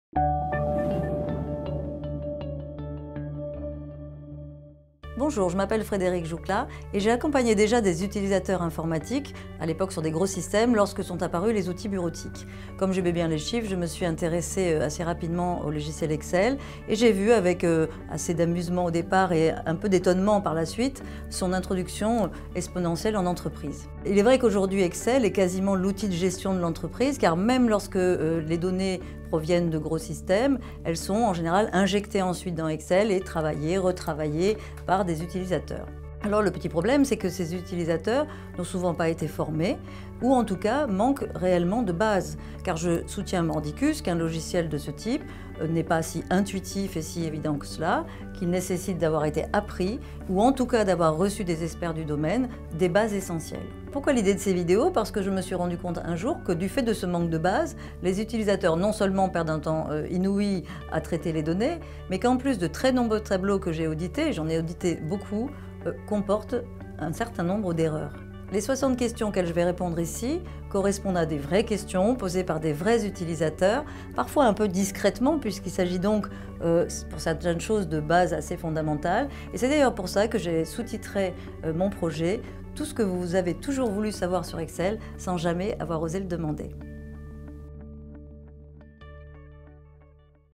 Formation vidéo 60 trucs et astuces Excel par Elephorm
Teaser du cours vidéo Elephorm 60 trucs et astuces Excel avec Frédérique Joucla.